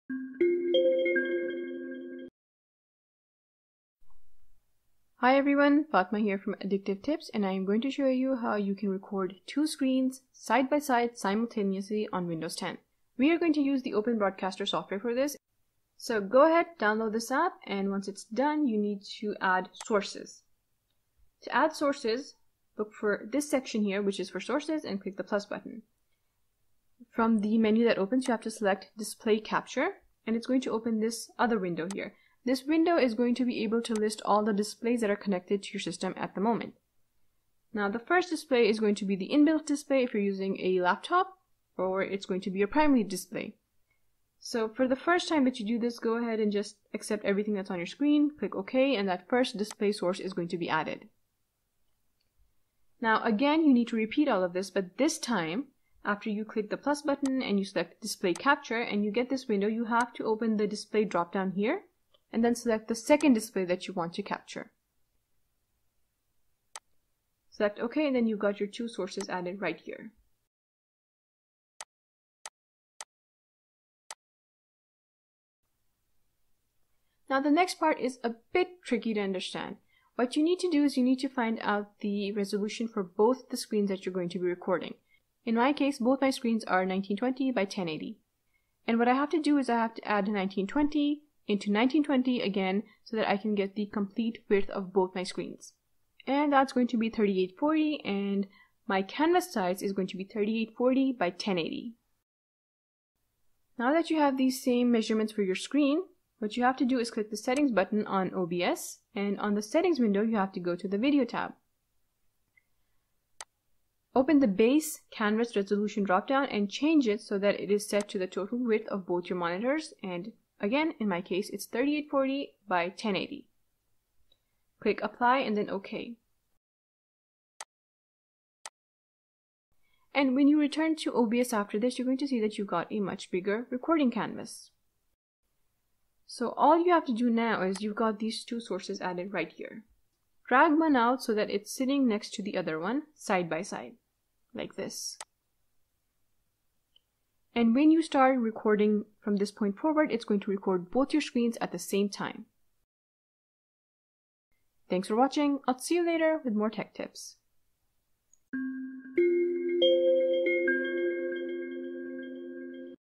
How to record multiple screens at once on Windows 10
Recording a PC screen has gotten easier. There are quite a few basic tools that allow you to record your screen. For more sophisticated needs, you can buy apps that do the job and make everything easy. If you need to record multiple screens at once though, you will find your options shrink.